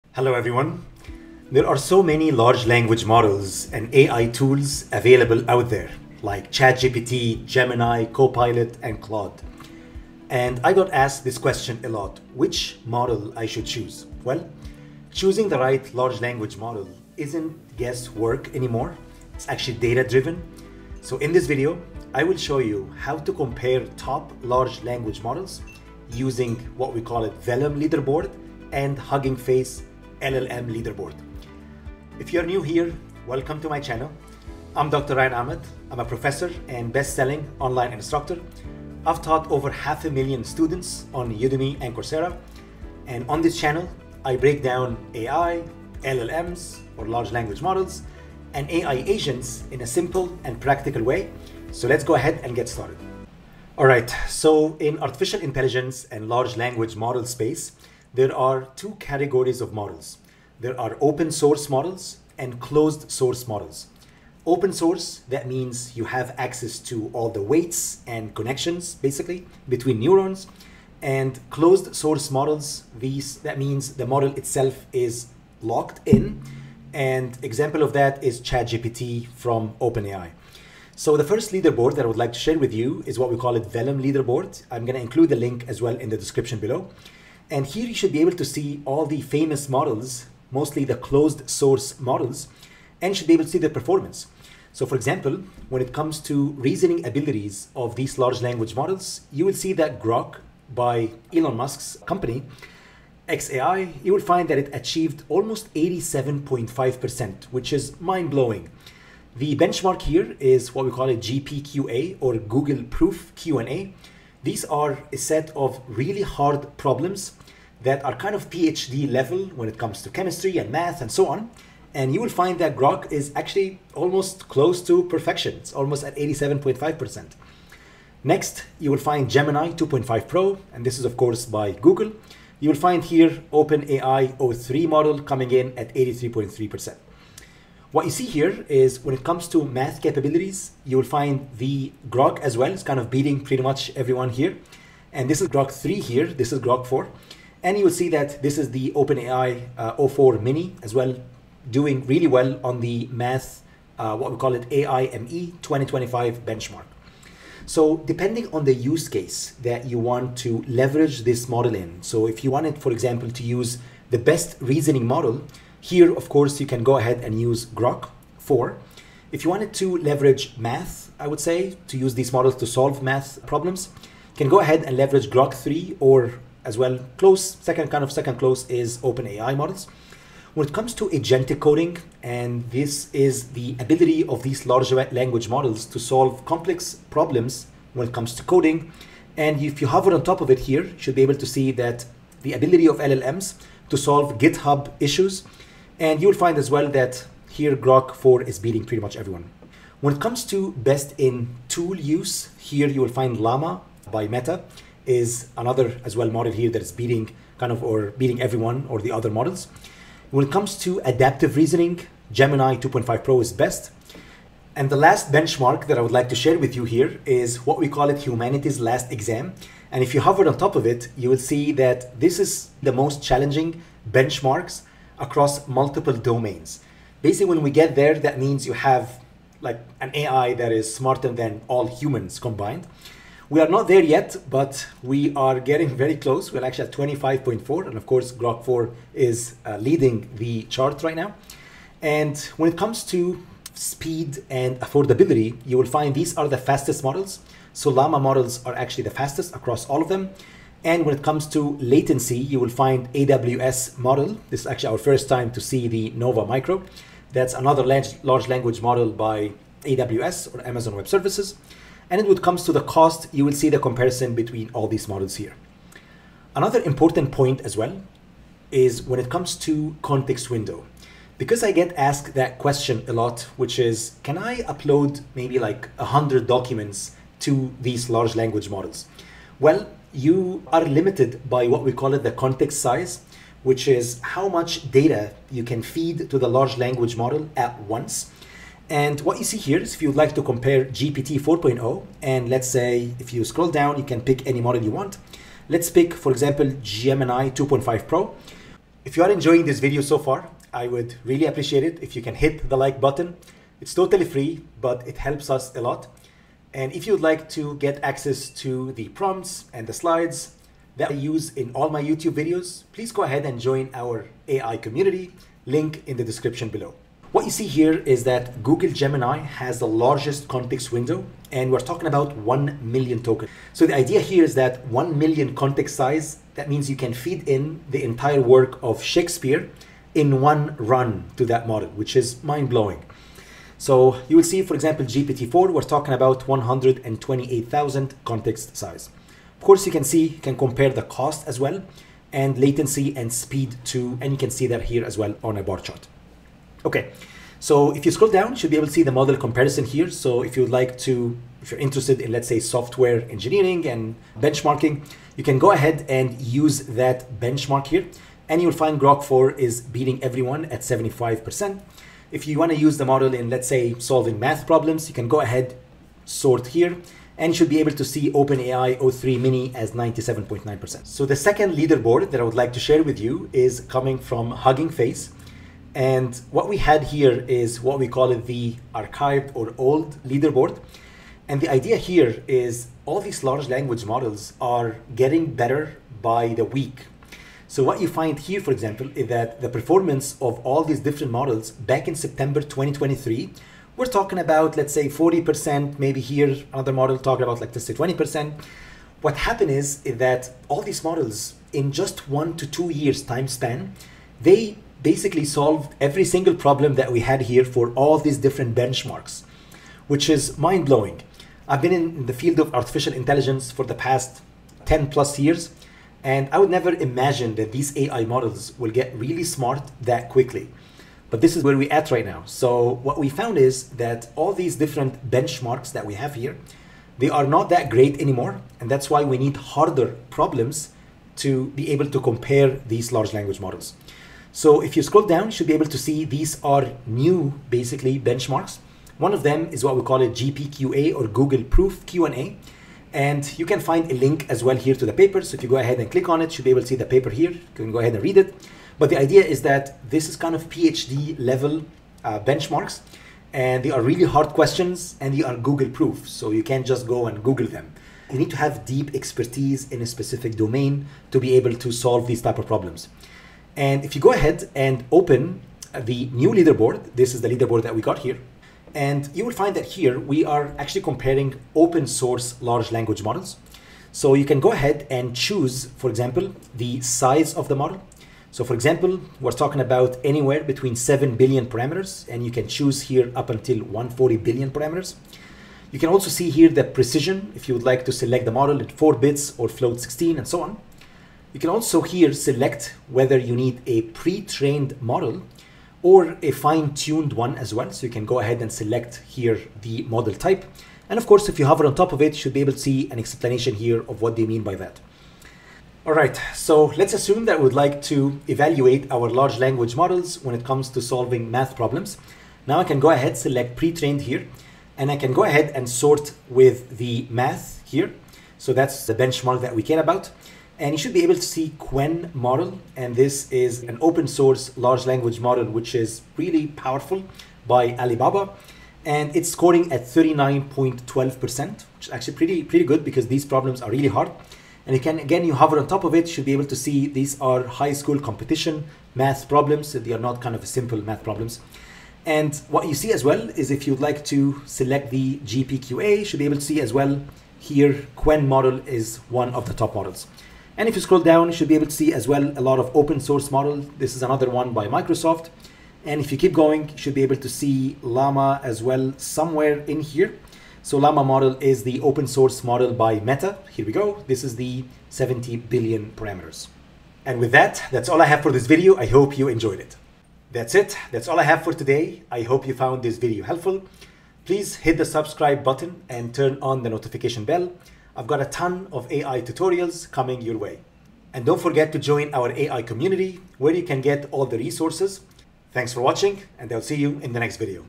Who’s Winning the AI Race? GPT-4o Vs. Gemini Vs. Grok Vs. Claude
🚀 Who’s Leading the LLM Race in 2025?
In this video, I break down the latest LLM leaderboard results and performance benchmarks for the top AI models — GPT-4o, Gemini, Grok, and Claude.

We’ll explore:
✅ Speed, latency, and context size comparisons
✅ Reasoning performance (GRIND benchmark)
✅ Cost, output, and real-world use cases
✅ Which model is best for YOU?

Whether you're an AI enthusiast, developer, or business leader this is your ultimate guide to understanding which large language model truly leads in 2025.